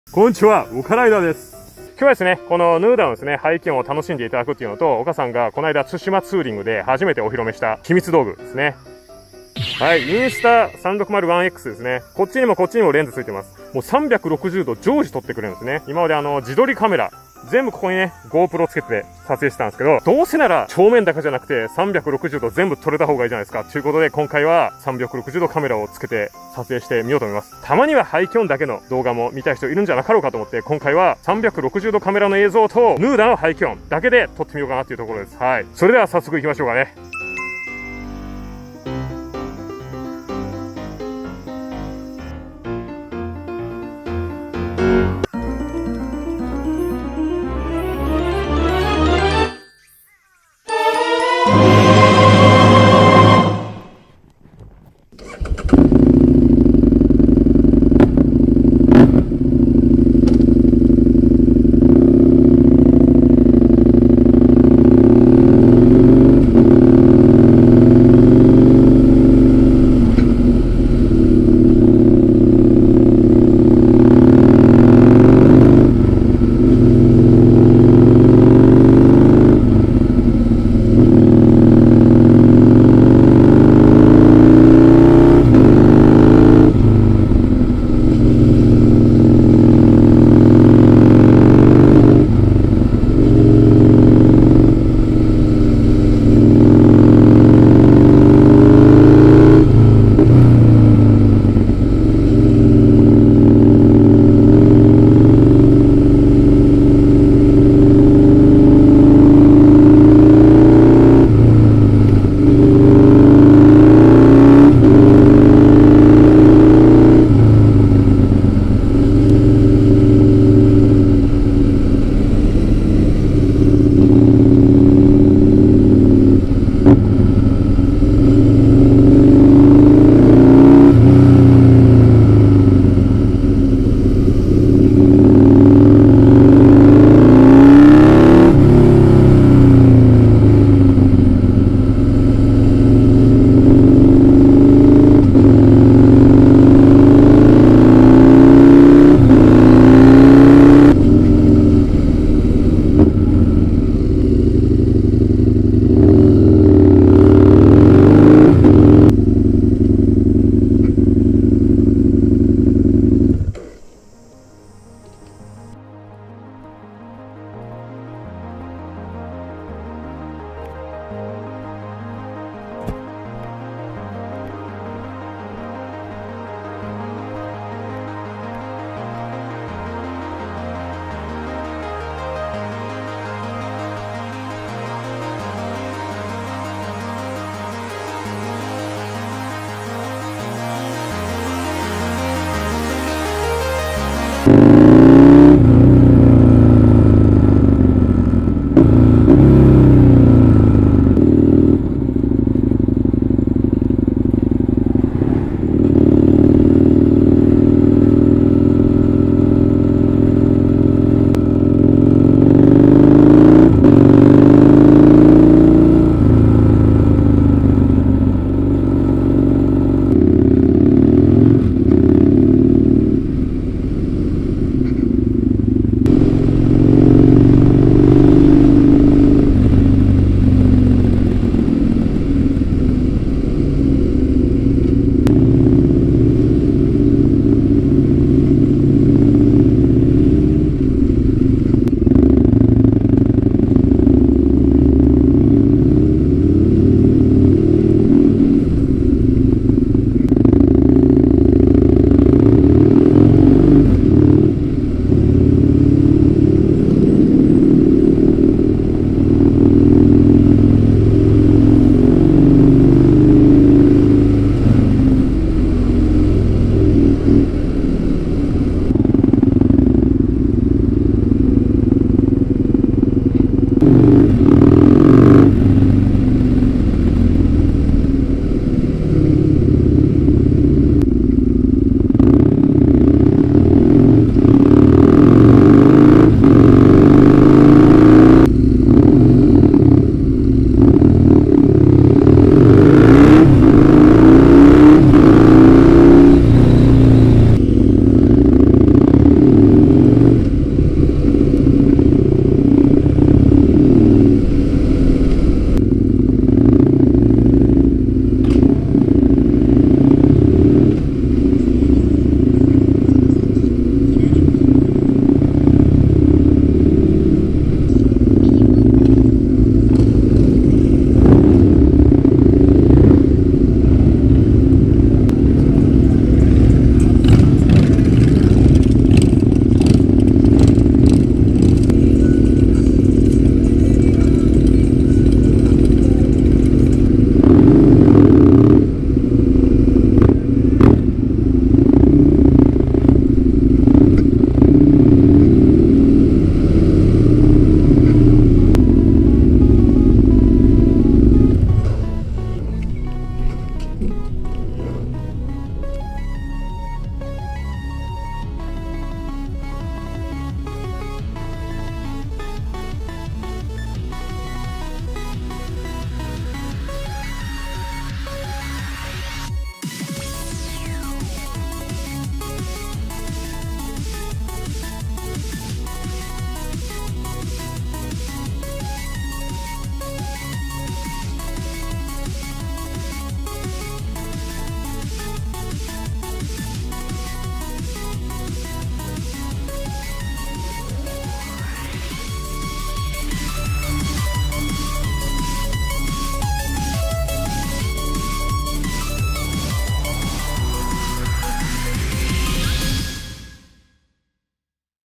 【極音ヌーダ】バイクの魅力の半分は排気音なのかもしれない。【Insta360 ONE X】
NUDA900Rはホントに良いバイク。もう2年乗ってますが飽きませんね！
もっと販売されてても良かったバイクですねコレは・・・
以下のリンクは僕が使っているinsta360 ONE Xのバイクセットが購入出来ます。